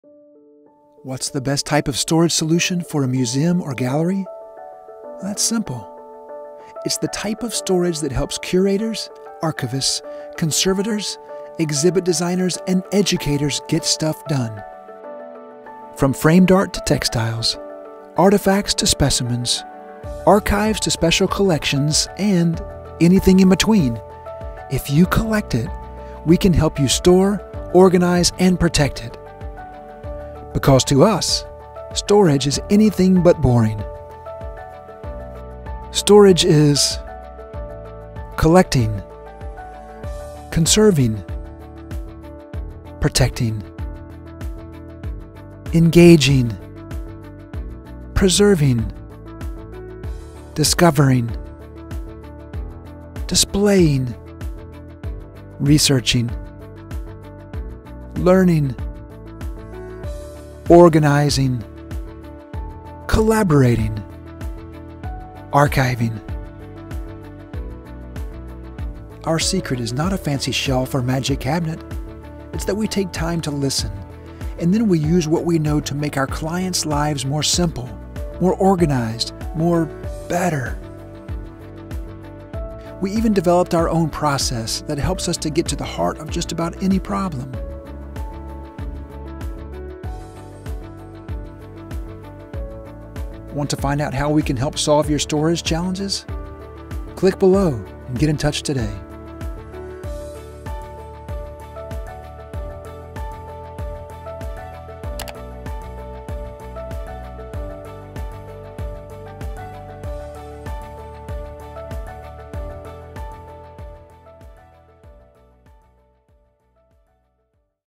Museum and Art Gallery Storage Solutions from Patterson Pope
What’s the best type of storage solution for a museum or gallery? That’s simple - it’s the type of storage that helps curators, archivists, conservators, exhibit designers and educators get stuff done. From framed art and textiles to artifacts and specimens to archives and special collection – and anything in between – if you collect it, we can help you store, organize and protect it. We provide a variety of museum storage products including museum cabinets, art rack, high-density compact shelving, modular casework, lockers and more.

Because to us, storage is anything BUT boring.